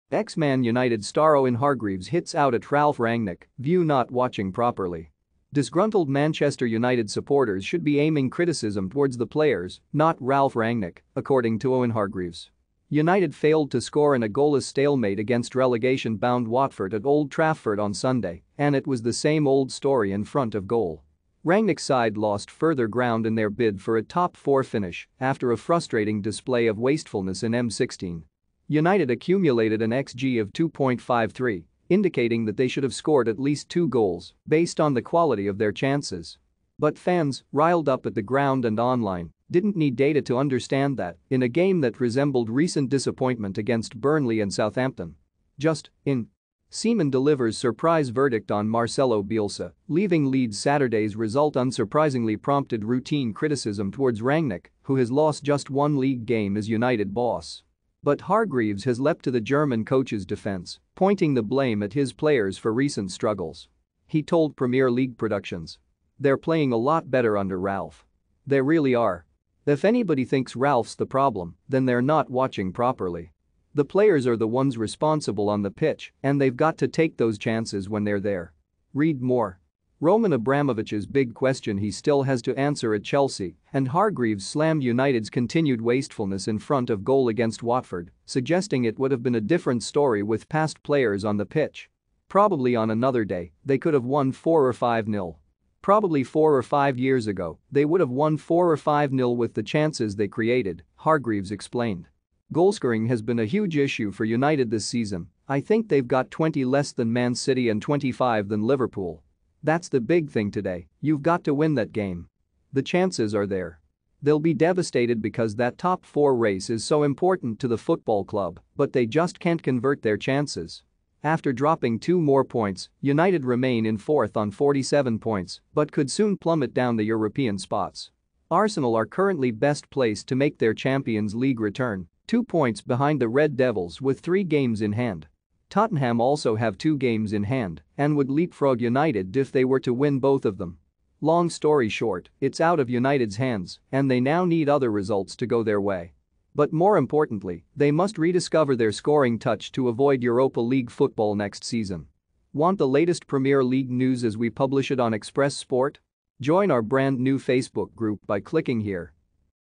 Ex-Man Utd star Owen Hargreaves hits out at Ralf Rangnick view - 'Not
Ex-Man Utd star Owen Hargreaves hits out at Ralf Rangnick view - 'Not watching properly'